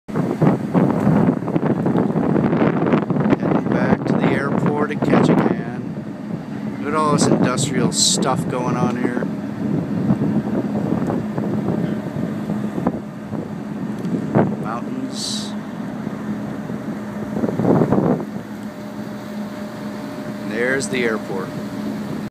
Ferry to Ketchikan Airport
A video from the ferry over to the Ketchikan Airport.  It's interesting that the airport actually on an island and you cannot drive to it to fly out of there...you have to take a boat...which which I guess is typical of Alaska in general.  I took this video while on a trip to Ketchikan and Sitka in May of 2012.